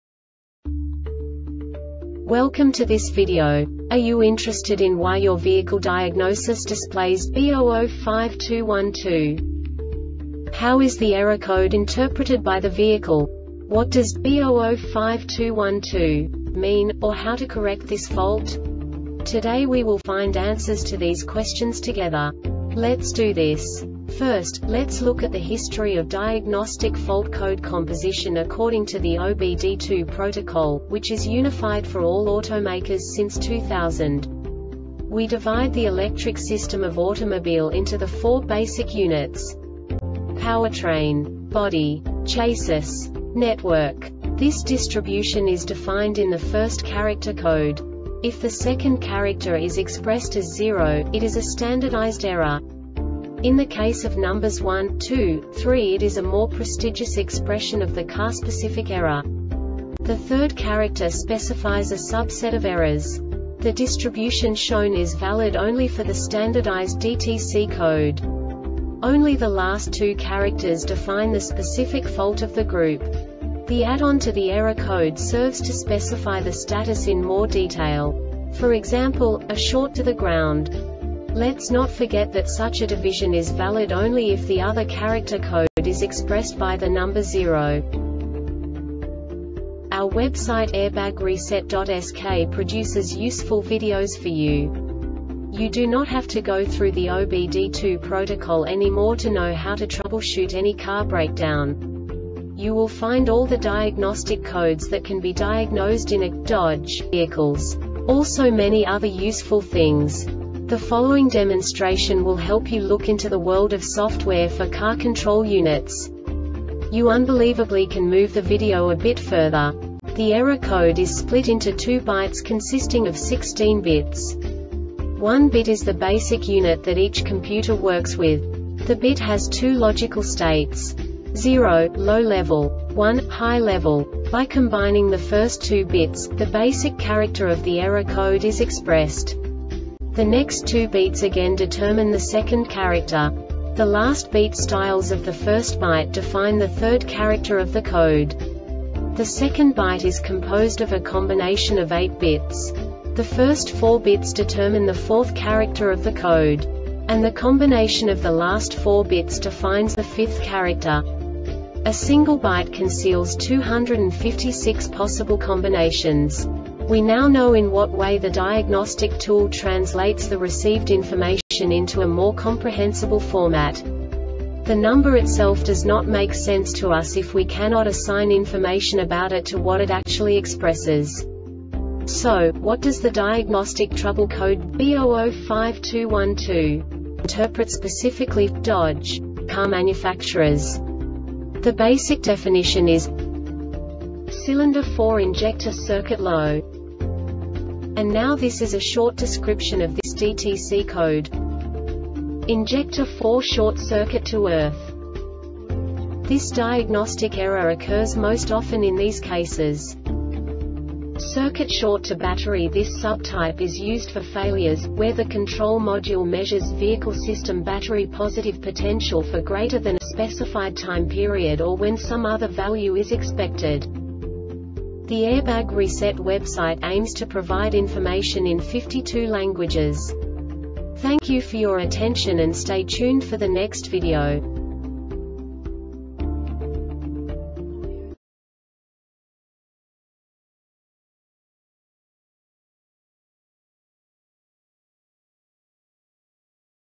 DTC Dodge B0052-12 Short Explanation
Contents:
0:21 Basic DTC analysis according to OBD2 protocol standard.
1:48 Insight into programming
2:58 A diagnostically understandable description of the Diagnostic Trouble Code
3:05 Basic error definition

Make
Dodge
Code
B0052 12
Definition
Cylinder 4 injector circuit low
Description
Injector 4 short circuit to earth
Failure Type